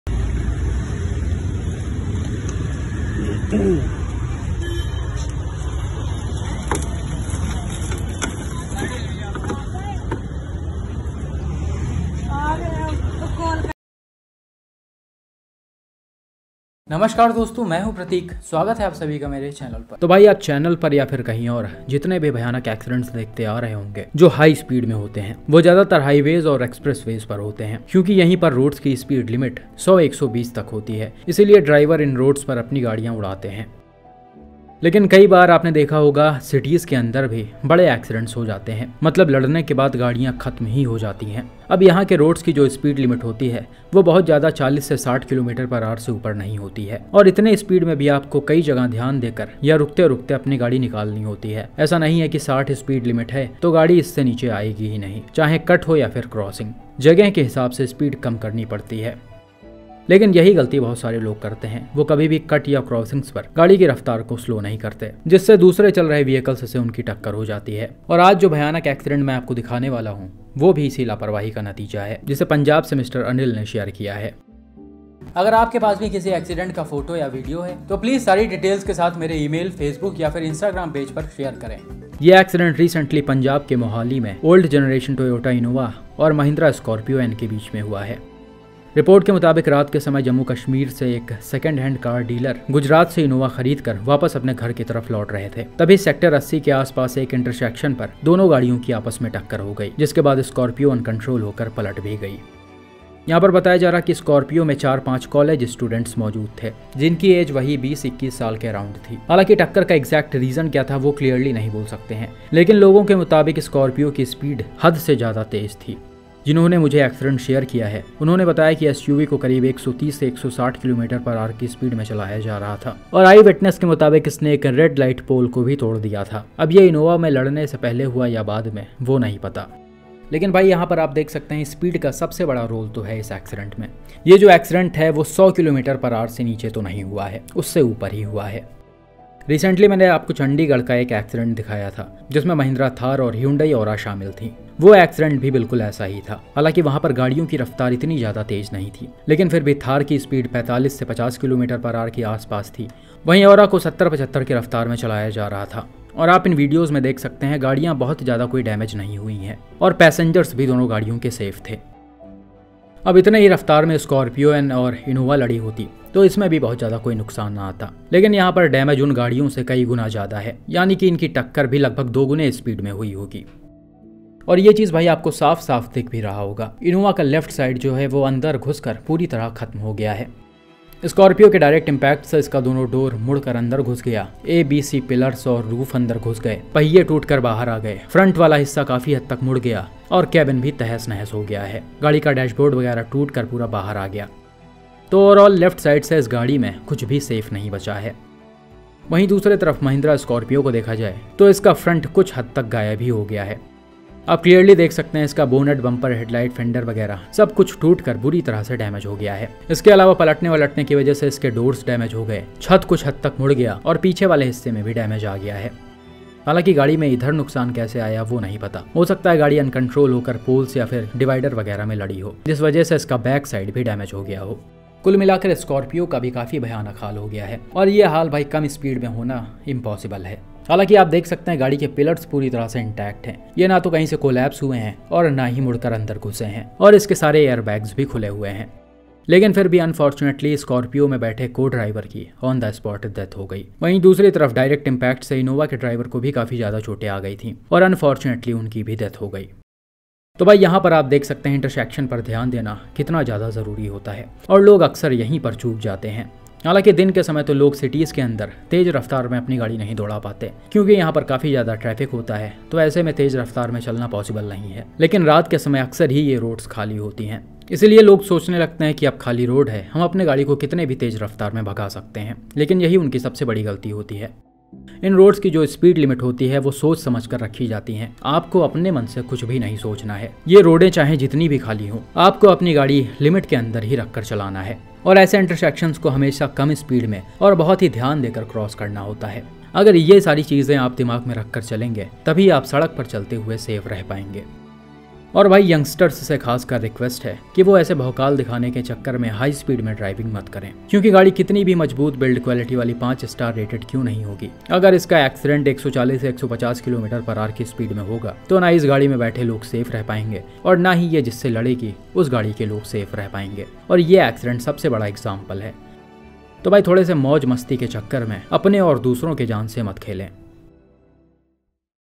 STUDENTS, SCORPIO N & 150 KMPH SPEED 😢 HORRIFIC RESULT OF RASH DRIVING
Recommended Dashcam
Price under 3k
Price 5k
Price 15k+ (using)
Price above 15k